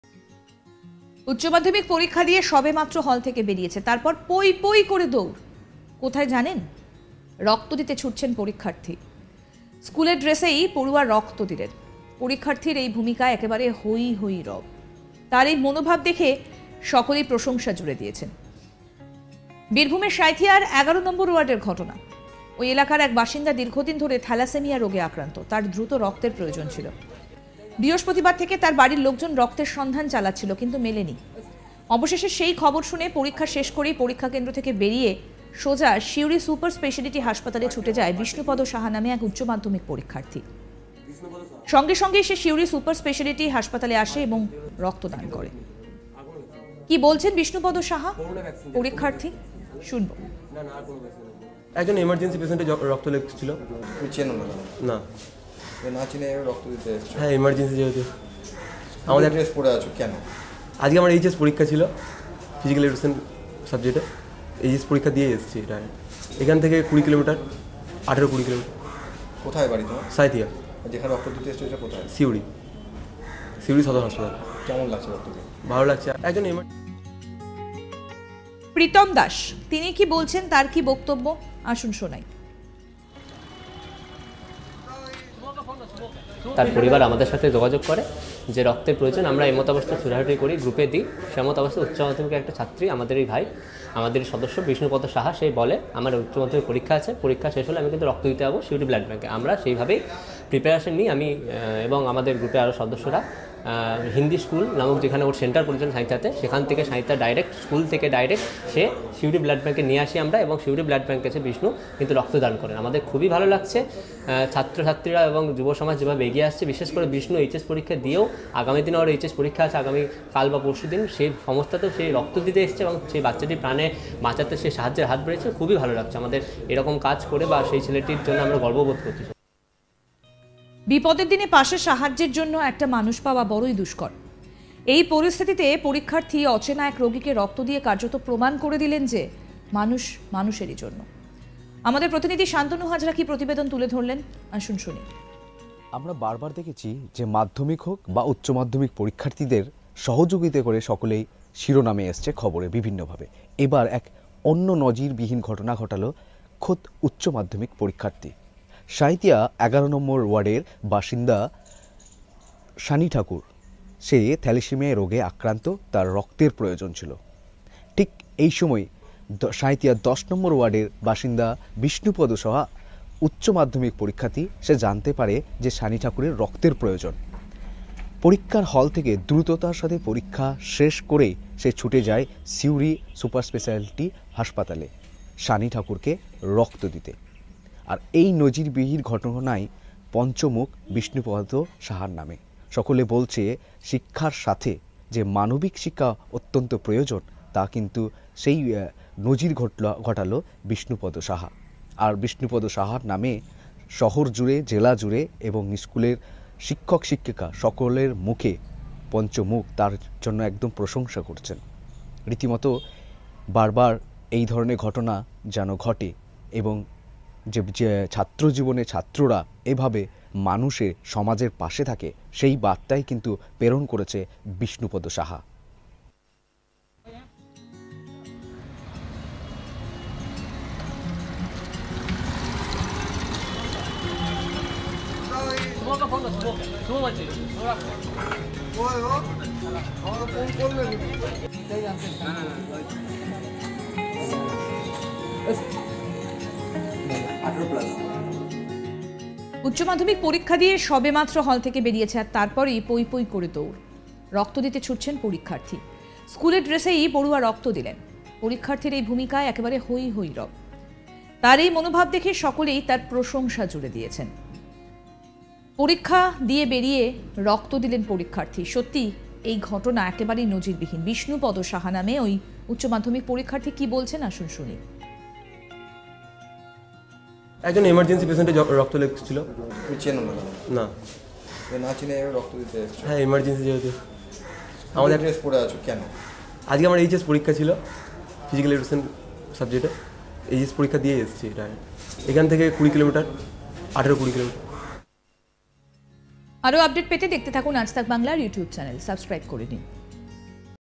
উচ্চমাধ্যমিক ছাত্র পরীক্ষা হল থেকে বেরিয়ে রোগীর জীবন বাঁচাল রক্ত দিয়ে| Birbhum News| Blood Donation
উচ্চ মাধ্যমিক পরীক্ষা দিয়ে সবেমাত্র হল থেকে বেরিয়েছে।
তারপরই পঁইপঁই করে দৌড়। কোথায় জানেন? রক্ত দিতে ছুটছেন পরীক্ষার্থী। স্কুলের ড্রেসেই পড়ুয়া রক্ত দিলেন।
পরীক্ষার্থীর এই ভূমিকায় একেবারে হইহই রব। তাঁর এই মনোভাব দেখে সকলেই তাঁর প্রশংসা জুড়ে দিয়েছেন।